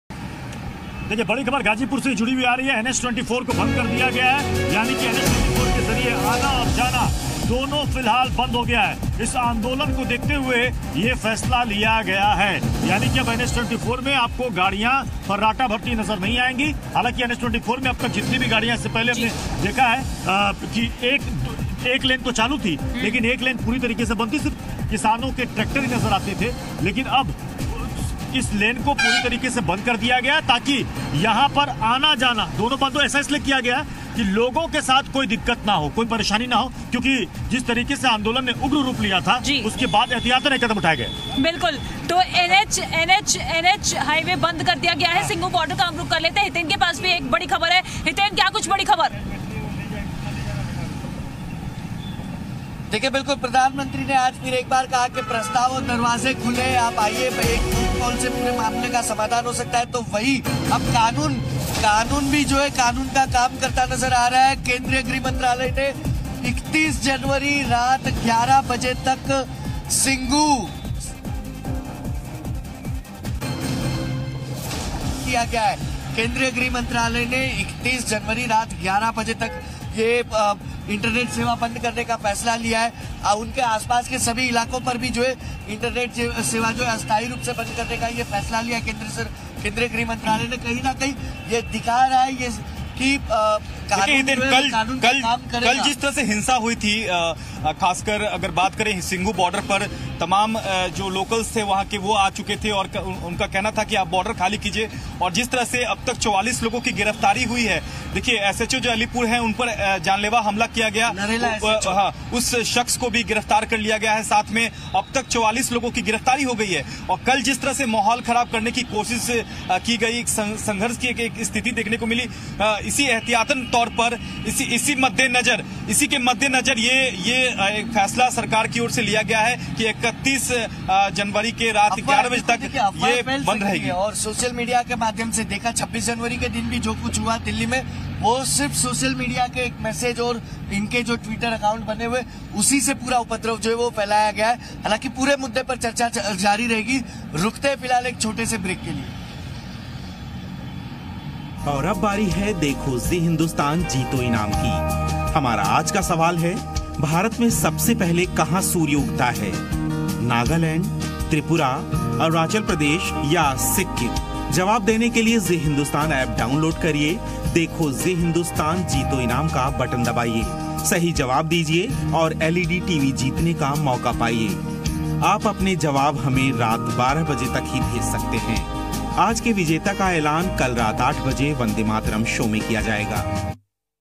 Kisan Andolan LIVE UPDATES | आंदोलन के चलते Police का बड़ा कदम, NH-24 को किया गया बंद
About Channel